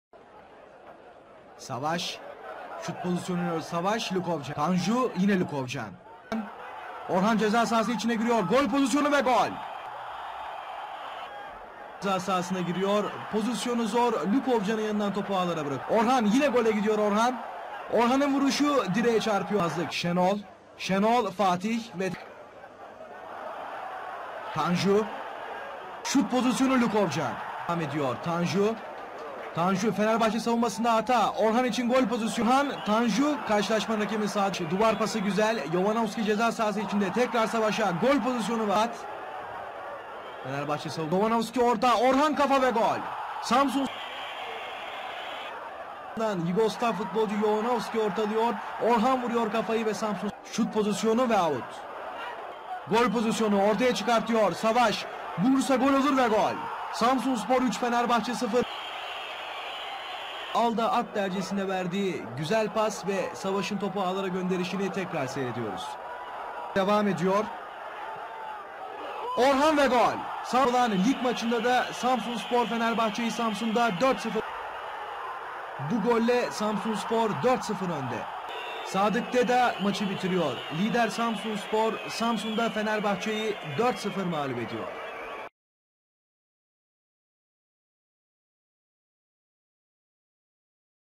1986 1987 Samsunspor Fenerbahçe UNUTULMAZ 11.Hafta Maçı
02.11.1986
samsunspor: fatih uraz, gijorgje (dorde) jovanovski, yaşar yiğit, muzaffer badalıoğlu, emin kar, savaş demiral, rıfat benli, ercüment coşkundere, erol dinler, orhan kapucu, tanju çolak

teknik direktör: milorad mitrovic

fenerbahçe: zivan ljukovcan, sedat karaoğlu, hasan özaydın (dk. 46 aykut yiğit), müjdat yetkiner, önder çakar, ismail kartal, hasan özdemir, kayhan kaynak (dk. 66 osman denizci), zafer tüzün, şenol çorlu, erdoğan yılmaz

teknik direktör: branko stankovic (yugoslavya)

gol:
dk. 40 orhan kapucu (1-0)
dk. 77 orhan kapucu (2-0)
dk. 83 savaş demiral (3-0)
dk. 88 orhan kapucu (4-0)

sarı kart:
dk. 61 gijorgje (dorde) jovanovski (samsunspor)
dk. 26 şenol çorlu, dk. 80 müjdat yetkiner (fenerbahçe)